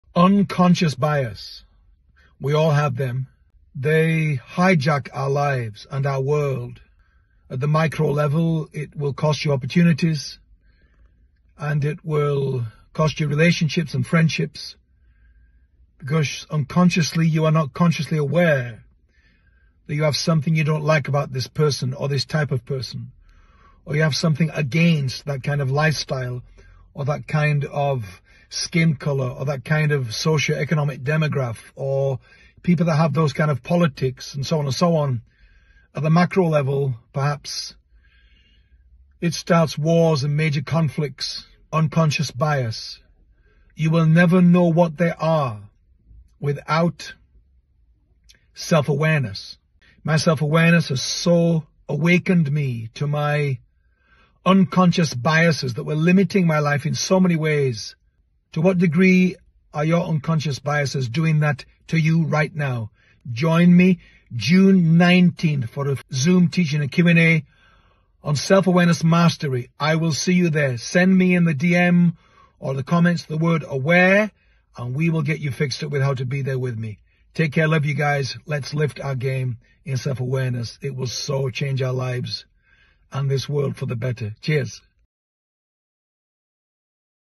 Stop Letting This Hijack Your Life!
Unconscious bias affects us all, shaping our perceptions and actions. It quietly influences our choices, hindering opportunities and connections.
Whether towards individuals or groups, these biases can unknowingly drive our decisions. By recognizing and addressing our own biases, we can break free from their hold.
Join me on June 19th for an insightful Zoom session on self-awareness mastery. Let's uncover and overcome these hidden barriers together.DM or comment "AWARE" to join the session. Elevate your self-awareness game with me.
Together, we can create a more inclusive and understanding world. See you there!